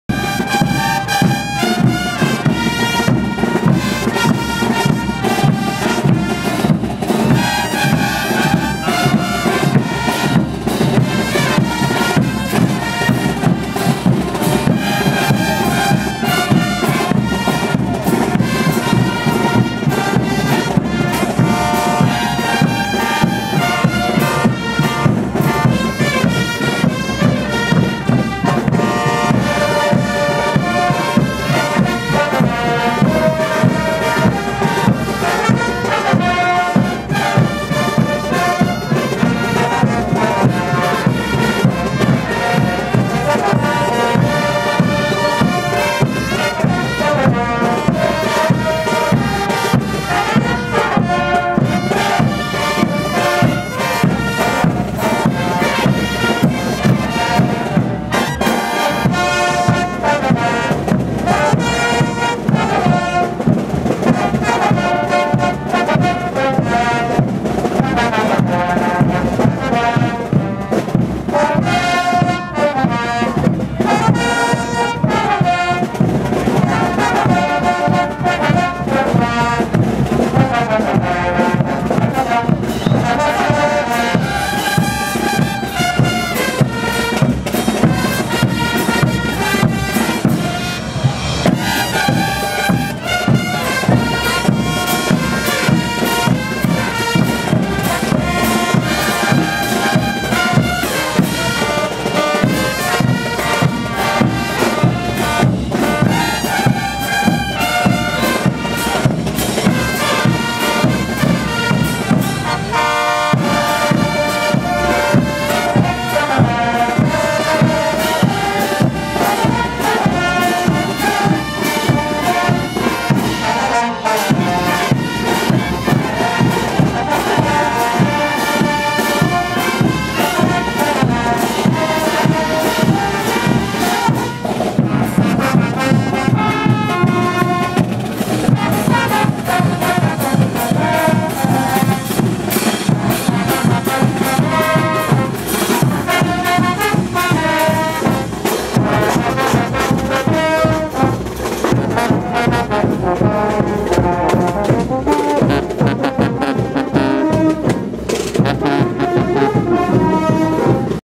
Regidor Carlos Huarca Castro bailando Morenada en la Plaza de Hunter
Regidor de la Municipalidad Distrital Jacobo Hunter  Carlos Alfredo Huarca Castro bailando Morenada con la Asoc. Cultural Morenada 3 de Abril,  San Juan Bautista  Amp. Pampas del Cusco;  y la poderosa y espectacular Banda Central Pagador  de Juliaca.